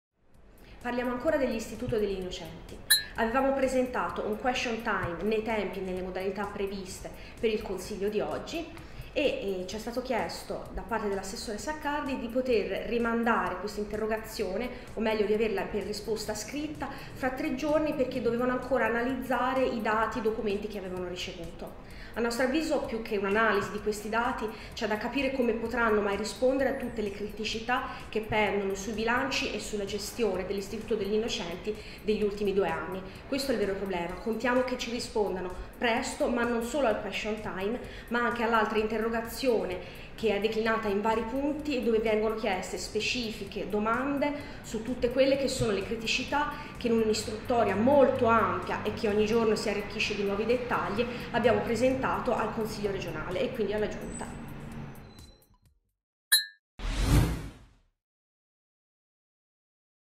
ISTITUTO INNOCENTI "SACCARDI PRENDE TEMPO, MA CARTA CANTA"
Irene Galletti, consigliera regionale M5S vicepresidente della Commissione Sviluppo Economico e Istruzione:

“Prendiamo atto che 14 giorni non sono bastati alla giunta Rossi per rispondere alle nostre domande sulla gestione dell’Istituto degli Innocenti e soprattutto a quella oggetto del question time: ovvero, alla luce dei fatti Rossi procederà al commissariamento? Imbarazza riconoscere quanto la Regione sia in difficoltà, non tanto a leggere i documenti nei tempi dati dal regolamento quanto a giustificarli, e questo prendere tempo è secondo noi chiaro segno della criticità del problema che abbiamo sollevato. Carta canta: la nostra interrogazione nasce da un’istruttoria precisa, arricchita di recente da nuovi dati altrettanto allarmanti. Il problema è grosso e la soluzione necessaria quanto prima, viste le modifiche allo Statuto richieste dall’attuale gestione”

"A questo punto chiediamo che come minimo l'assessora risponda non solo al question time ma anche all'interrogazione dettagliata che abbiamo presentato a integrazione del question time"

Interrogazione Orale urgente del Movimento 5 Stelle disponibile al seguente